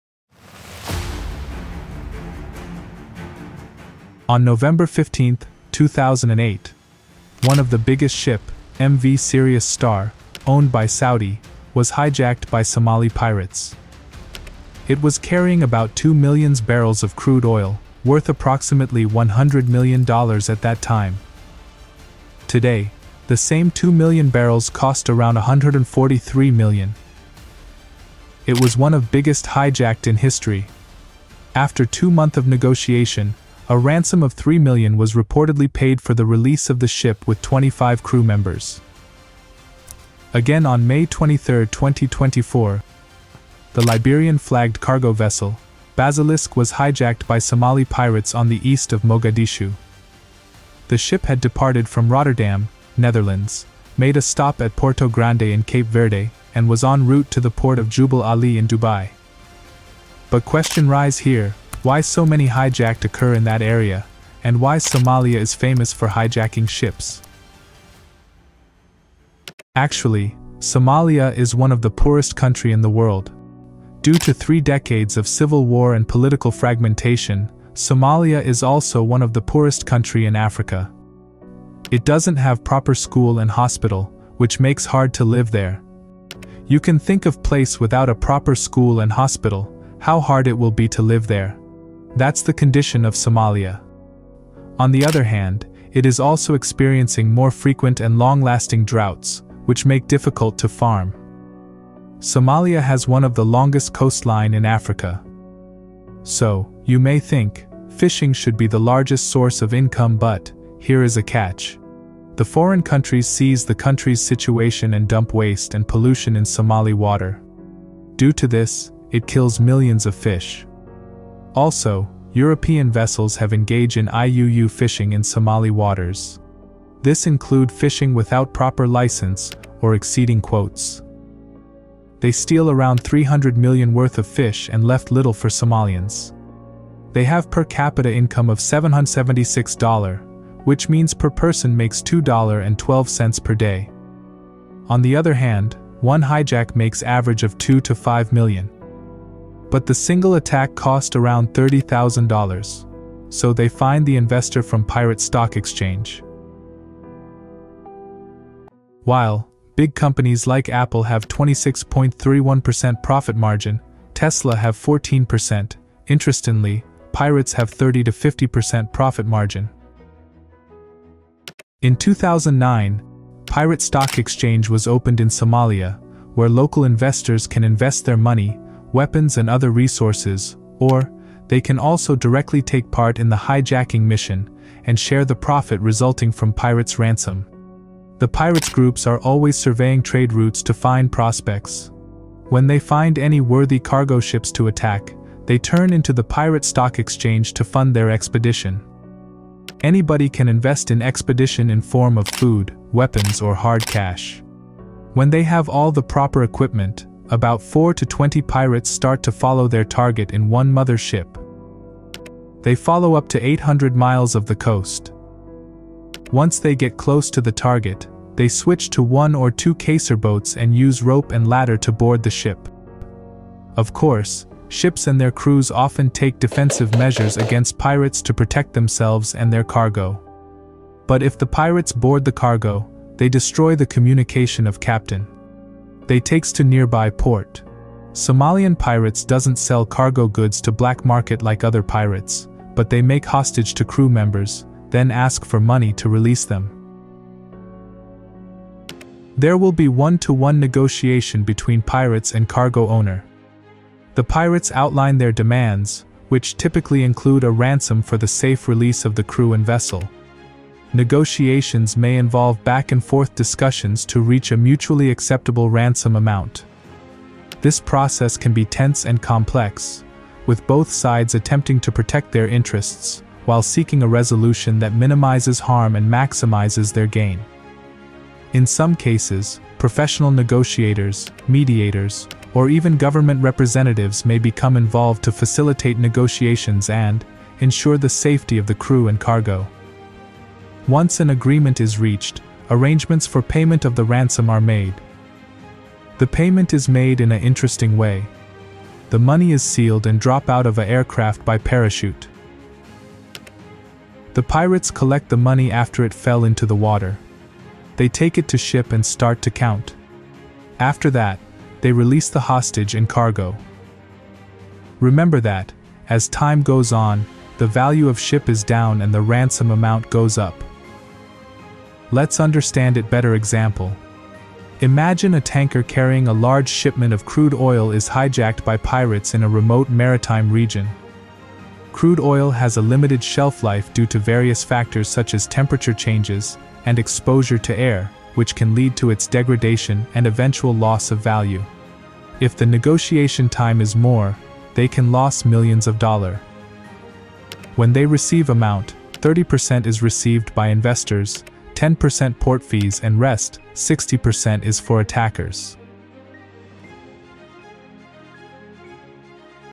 How Somali Pirates have better profit margins than Apple? Explained
Somalia is one of the poorest country in the world but this country is famous for pirates. Many ships and cargo has been hijacked by Somali Pirates and release them after they get some money. how Somali pirates makes millions, let's have a look in video.
Research: Bikash
Voice: John
Edit: Bikash